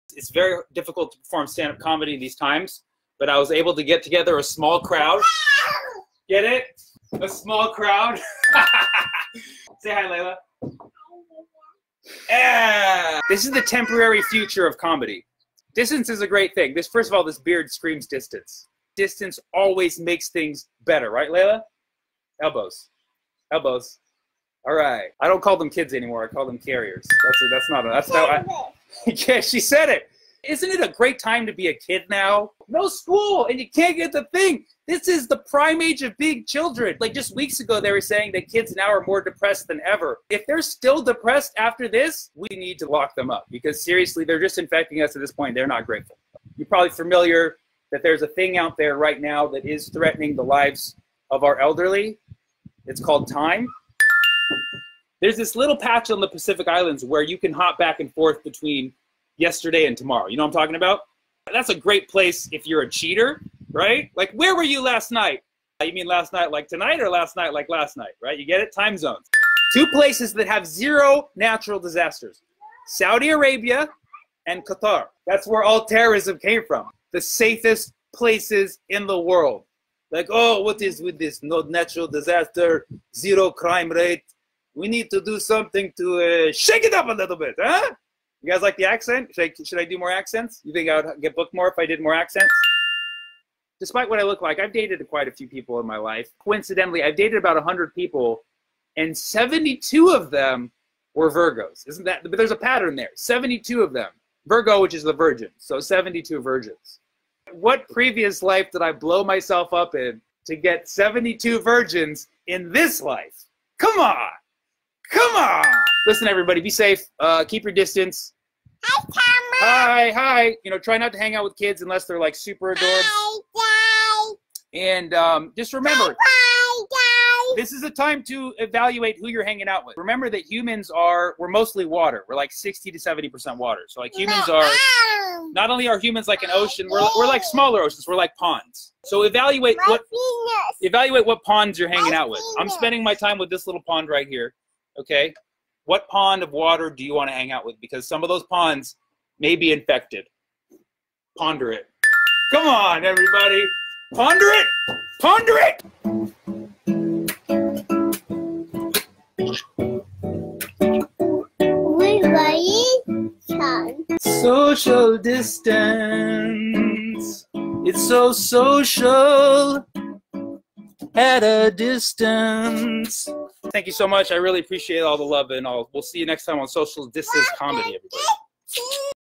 Social Distance Comedy
Despite the lockdown, I was able to perform for a small crowd in Maui